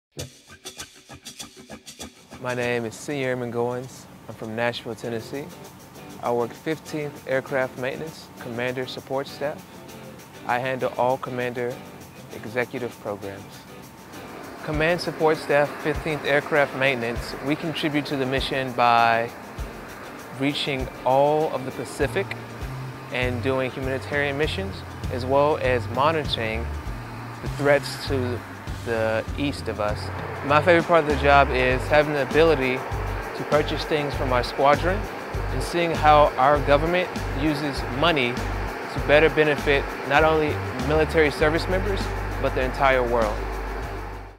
Airman Spotlight: SrA James Goins
Nashville native, SrA James Goins, helps ensure everything runs smoothly in Commander Support Staff in this week's Airman Spotlight!

(U.S. Air Force Video by Senior Airman Michael Reeves Jr.)